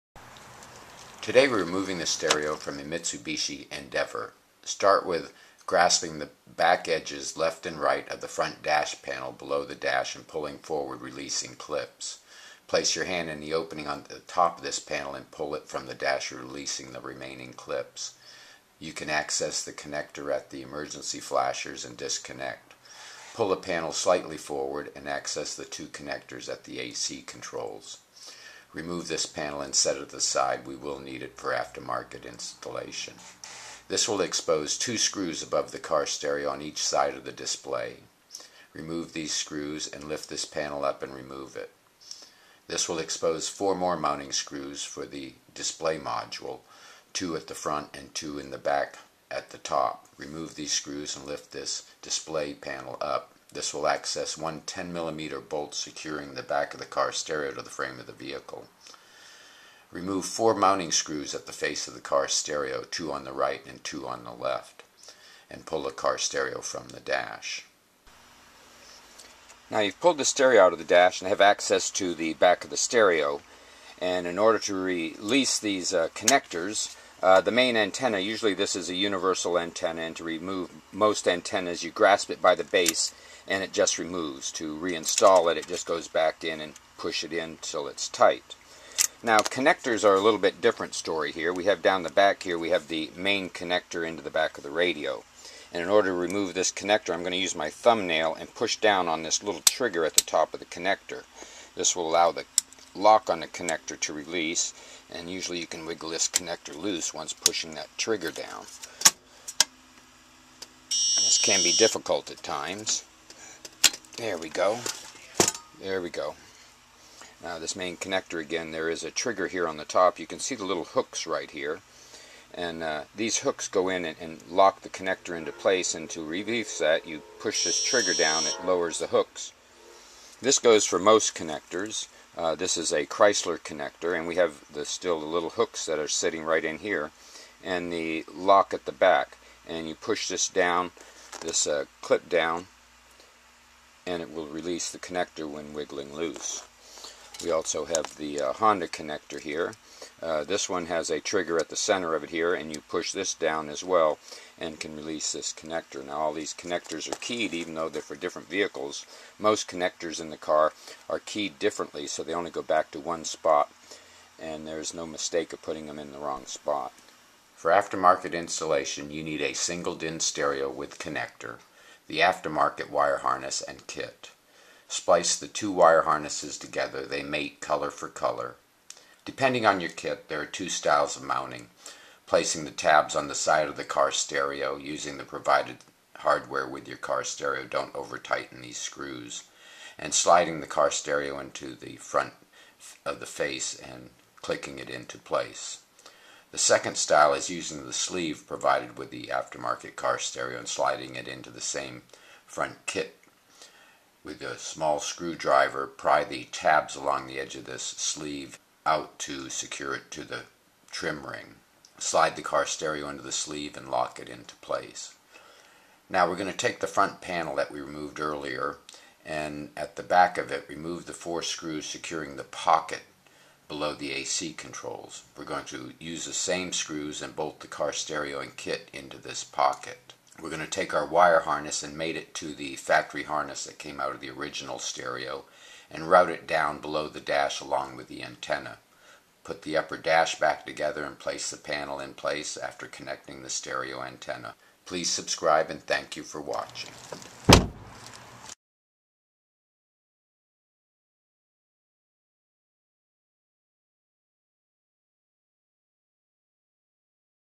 Mitsubishi Endeavor Car Stereo Removal and Installation = Car Stereo HELP
Car Stereo Help ! Mitsubishi Endeavor Car Stereo Removal and Aftermarket Radio Installation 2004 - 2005. - Car stereo, speaker, amplifier an antenna - remove, install, troubleshoot, replace and repair.